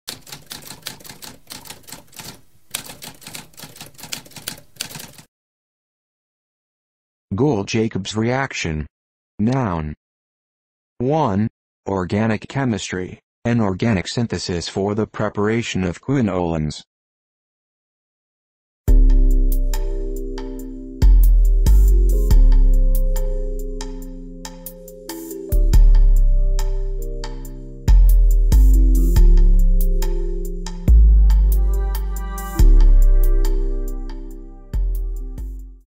What does Gould-Jacobs reaction mean?
A spoken definition of Gould-Jacobs reaction.

Intro Sound:
Typewriter - Tamskp